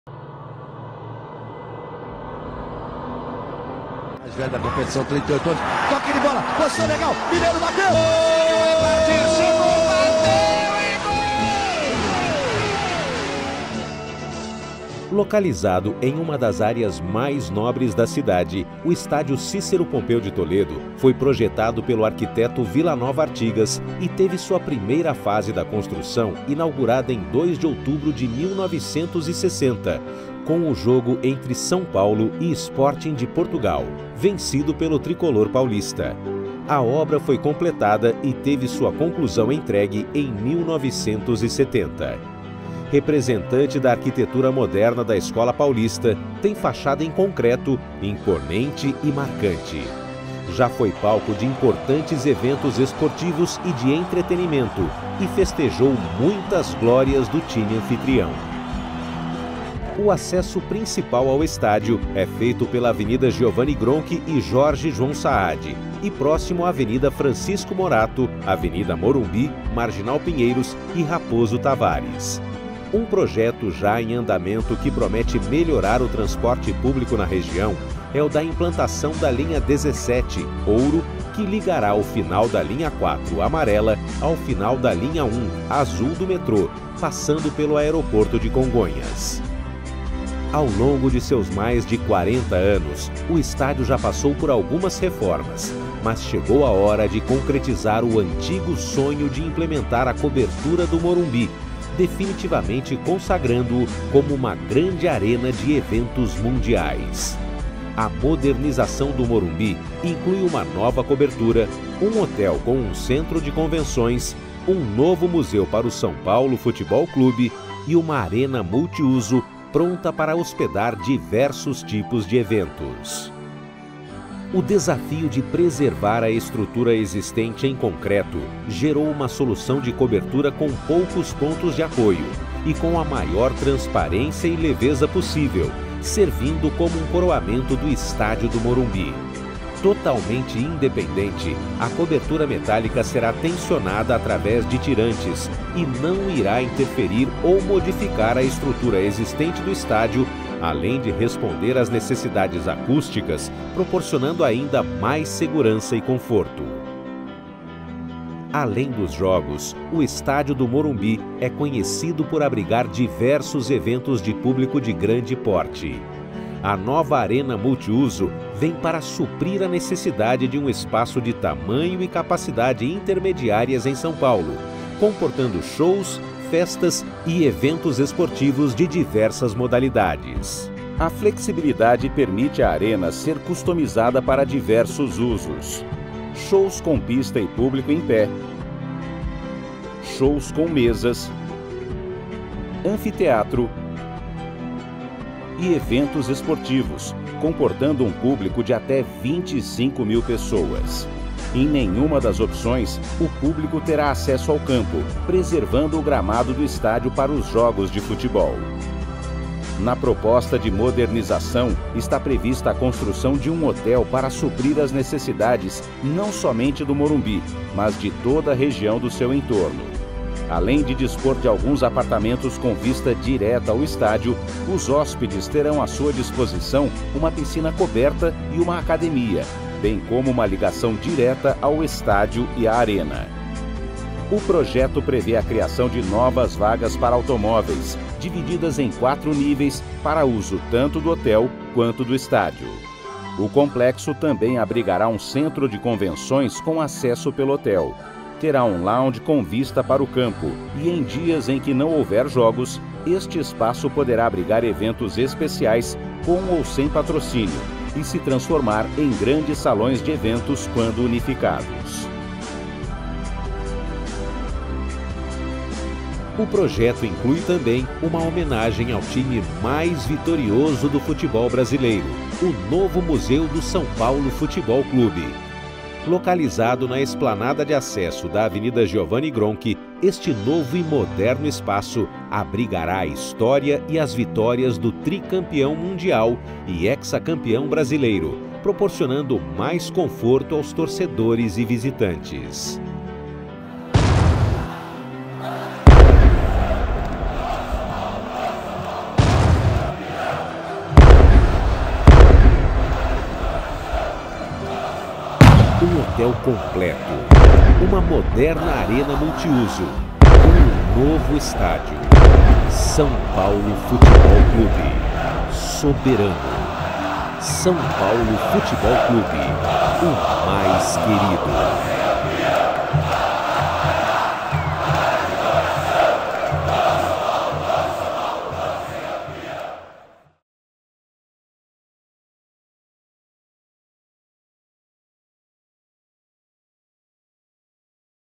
Modernização do Estádio do Morumbi
Projeto de modernização do Estadio Cicero Pompeu de Toledo, O Morumbi em parceria com a Andrade Gutierres.